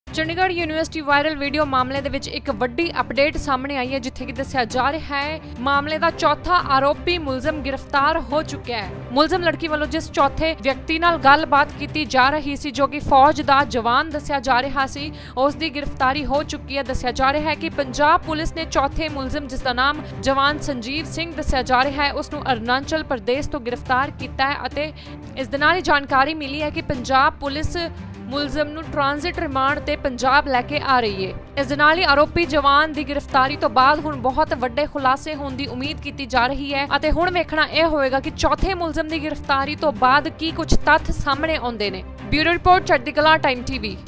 ਚੰਡੀਗੜ੍ਹ ਯੂਨੀਵਰਸਿਟੀ ਮਾਮਲੇ ਦਾ ਚੌਥਾ ਆਰੋਪੀ ਗ੍ਰਿਫਤਾਰ
* ਪੰਜਾਬ ਪੁਲਿਸ ਨੇ ਆਰੋਪੀ ਜਵਾਨ ਸੰਜੀਵ ਸਿੰਘ ਨੂੰ ਕੀਤਾ ਗ੍ਰਿਫਤਾਰ
* ਅਰੁਣਾਚਲ ਪ੍ਰਦੇਸ਼ ਤੋਂ ਕੀਤਾ ਗਿਆ ਗ੍ਰਿਫਤਾਰ
* ਆਰੋਪੀ ਨੂੰ ਟਰਾਂਜਿਟ ਰਿਮਾਂਡ ’ਤੇ ਪੰਜਾਬ ਲਿਆ ਰਹੀ ਹੈ ਪੰਜਾਬ ਪੁਲਿਸ
* ਫ਼ੌਜ ਦਾ ਜਵਾਨ ਚੌਥਾ ਅਪਰਾਧੀ ਵੀ ਹੋ ਗਿਆ ਗ੍ਰਿਫਤਾਰ